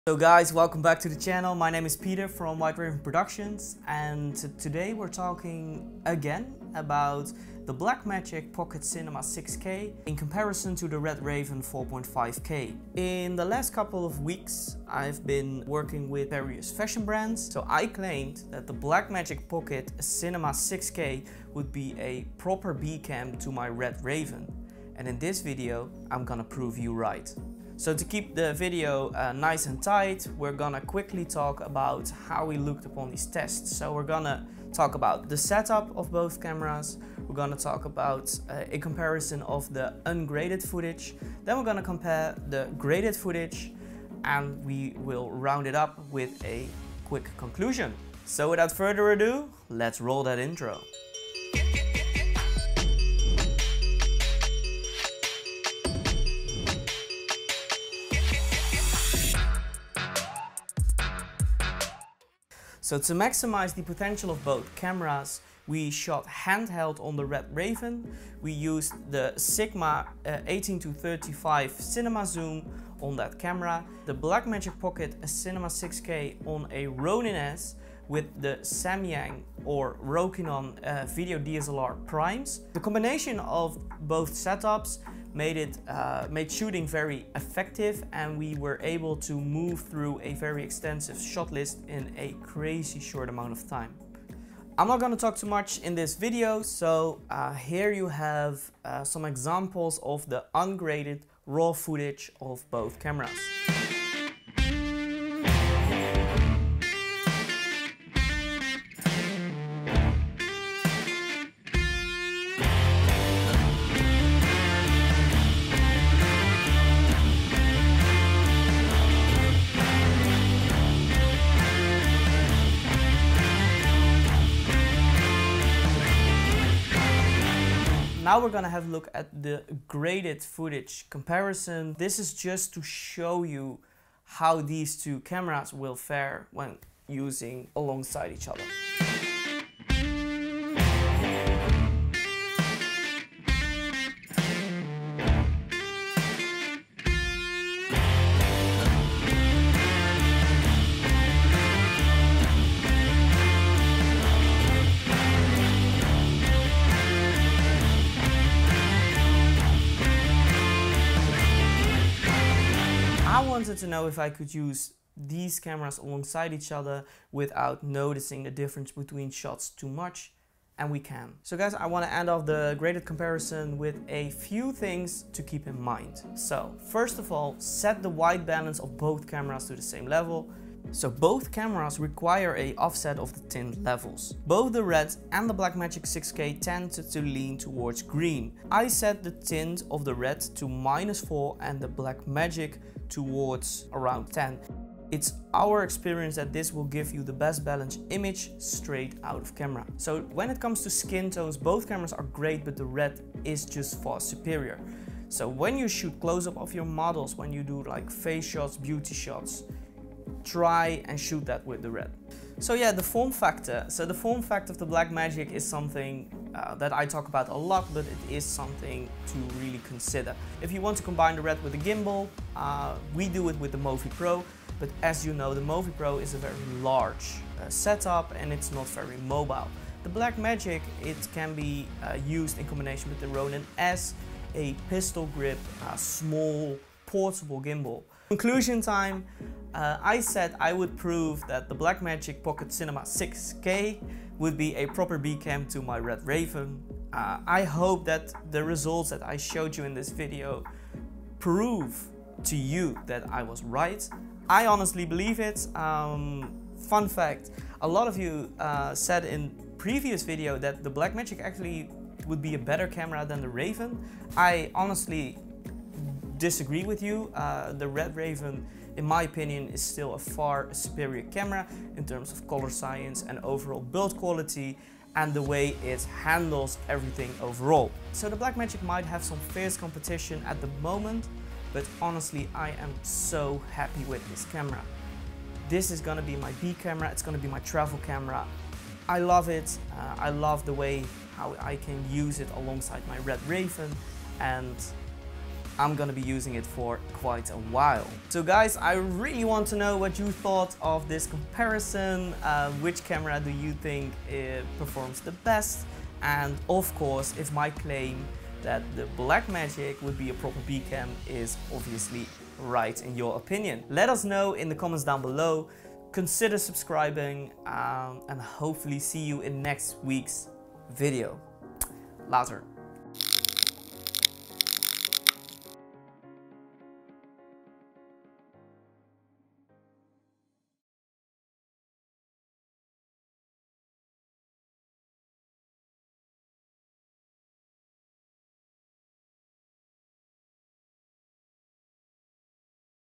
RED Raven 4.5k & Blackmagic pocket 6k - how we use them in real life!
Welcome back! This week's video is all about real life tests. We've been using the red for a long time now and we want to know if we can use the bmpcc 6k as a b-cam.  Today I’ll show you that the BMPCC is perfect as my b-cam.